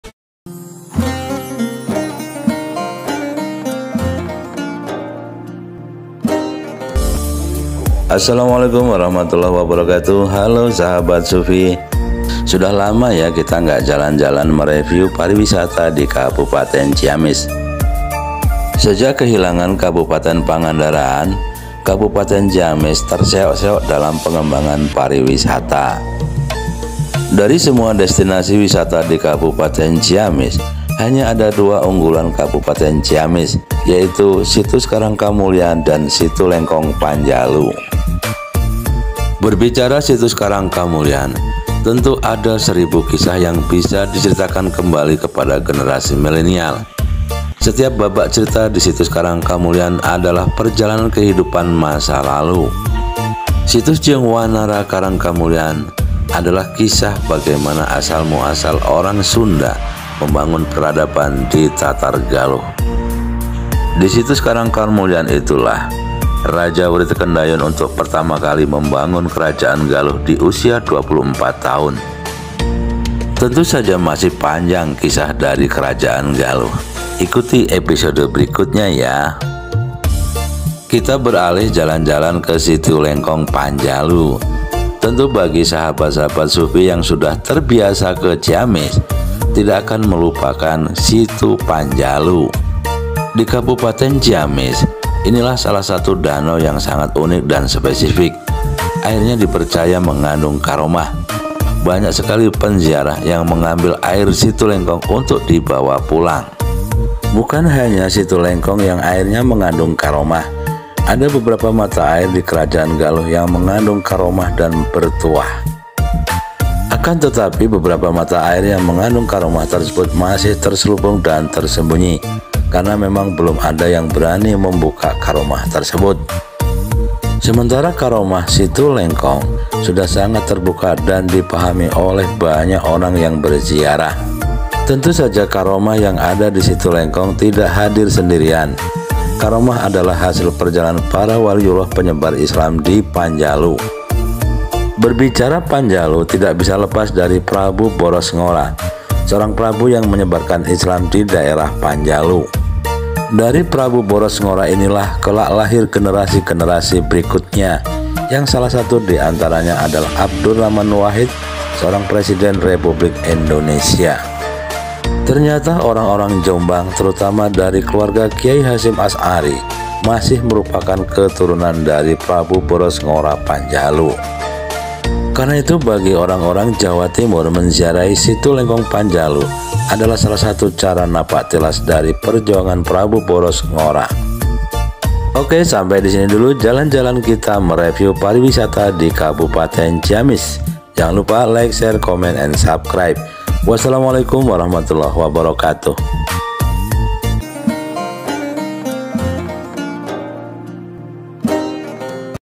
Wisata Minat Khusus, Napak Tilas Sejarah di Situs Karangkamulyan dan Situ Lengkong Panjalu
Ini jalan jalan mereview wisata di Ciamis. Yuks ikuti seperti apa wisata unggulan di Ciamis.